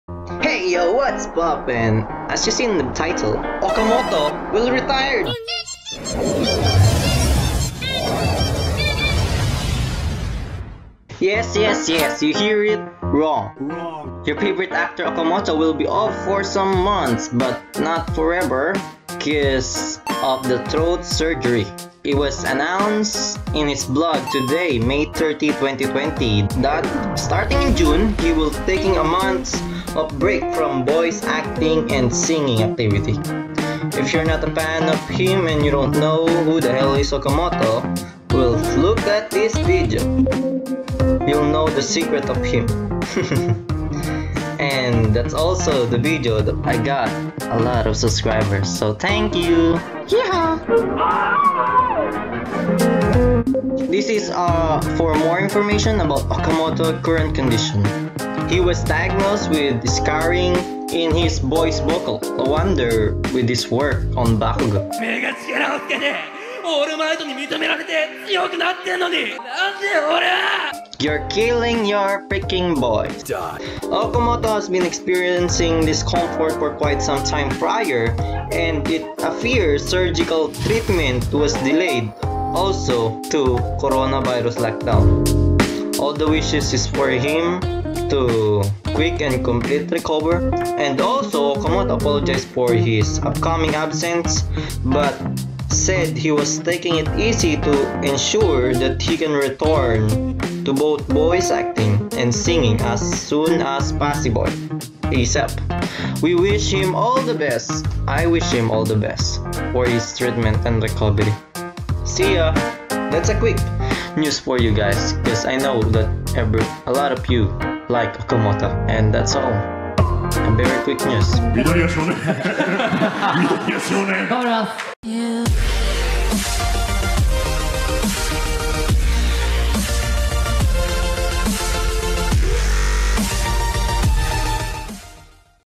Voice Actor Nobuhiko Okamoto Takes Retard becaese of a big problem
Your favorite actor Okamoto will be off for some months of break becease of Throat Surgery. It's announced on his blog today (May 30, 2020) that, starting in June, he will be taking a month's hiatus from voice acting and singing
activities.

All the best wishes for a quick and complete recovery!

Okamoto apologized for his upcoming absence, but said he will be taking it easy to ensure that he can return to both voice acting and
singing as soon as possible. We wish him all the best with his treatment and recovery.